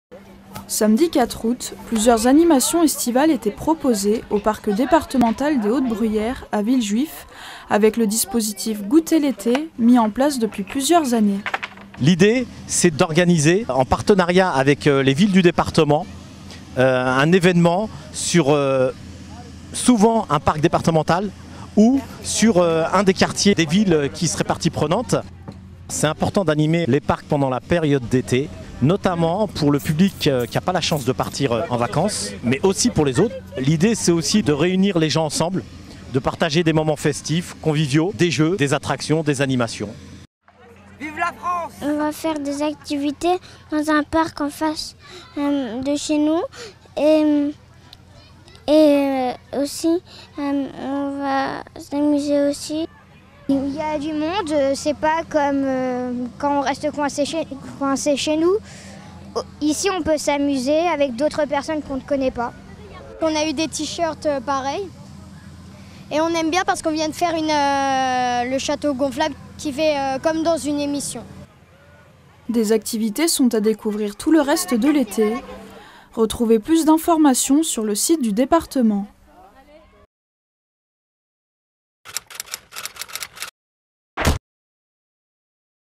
Goutez l'été continue au parc départemental des hautes bruyères, à Villejuif
Ce samedi 5 août, malgré les fortes chaleurs, les animations reportées après le pic de température ont réjoui petits et grands pour un moment convivial jusqu'à la séance de cinéma en plein air.

📎 Retrouvez toute la programmation de « Goutez l’été », une opération proposée par le Département du Val-de-Marne en partenariat avec les villes et les associations locales, jusqu'à fin août,